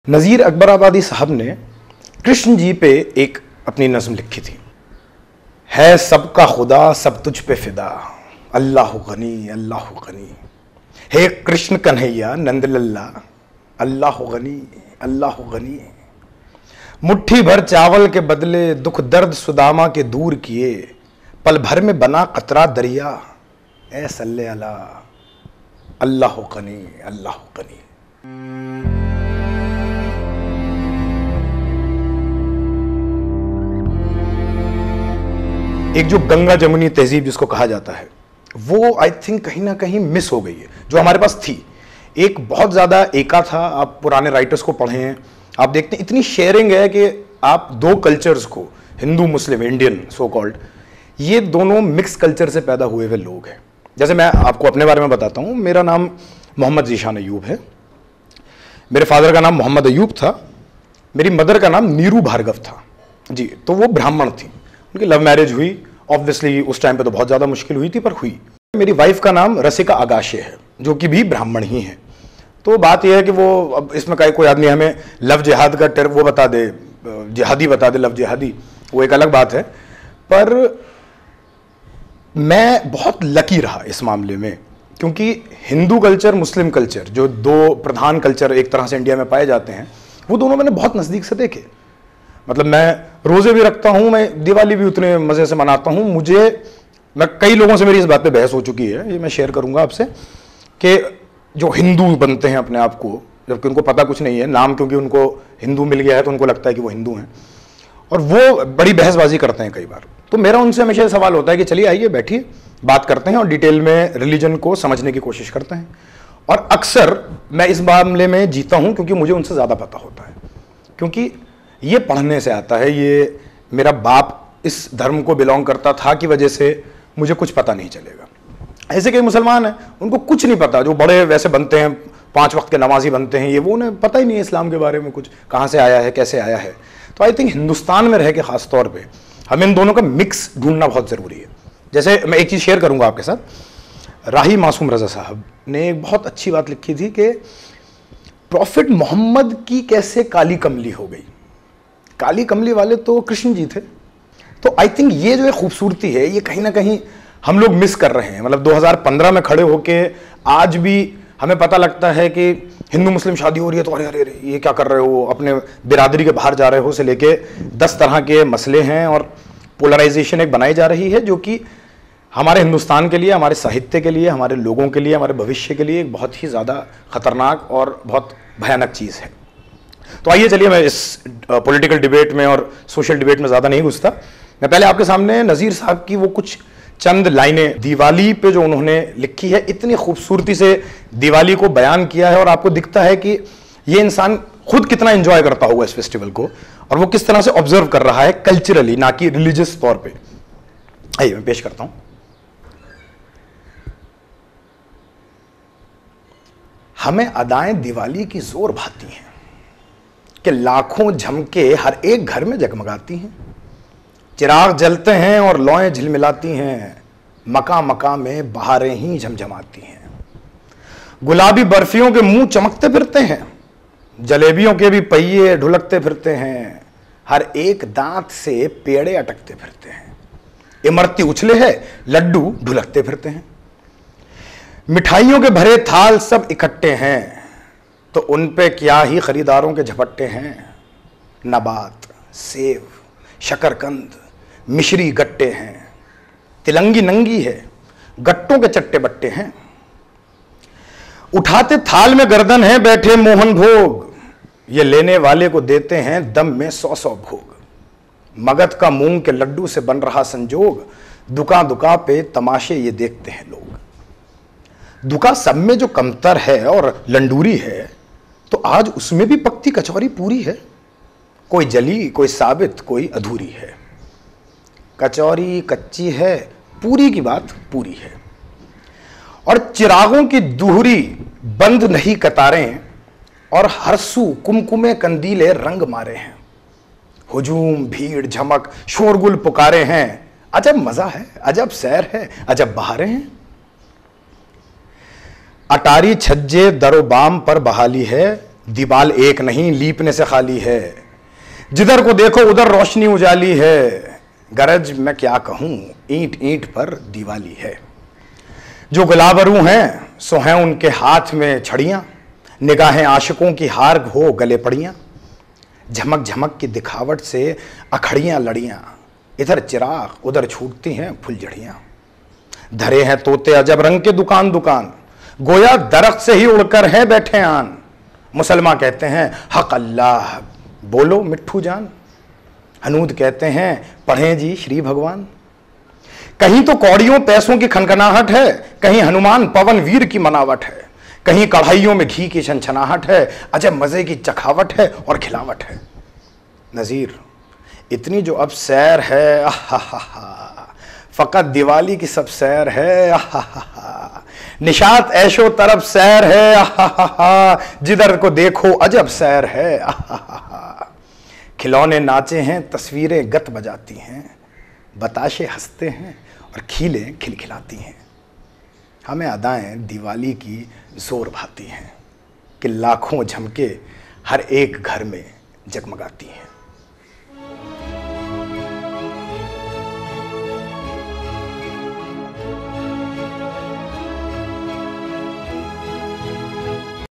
Hindi Studio : Nazeer Akbarabadi : Diwali : Zeeshan Ayub with Manish Gupta
Nazeer Akbarabadi (Urdu: نظیر اکبر آبادی‎ – Naẓīr Akbar Ābādī) (real name "Wali Muhammad") (1735–1830) was an 18th-century Indian poet known as "Father of Nazm", who wrote Urdu ghazals and nazms under nom de plume (takhallus) "Nazeer", most remembered for his poems like Banjaranama (Chronicle of the Nomad), a satire.[1] His father was Muhammad Farooq and his mother was the daughter of Nawab Sultan Khan who was the governor of Agra Fort. Agra, the Indian city, was known as Akbarabad after Mughal emperor Akbar at that time. He used simple, everyday language in his poems, which made them popular in the masses.

© एक्टिव Illusions
Created by : Manish/Malvika Gupta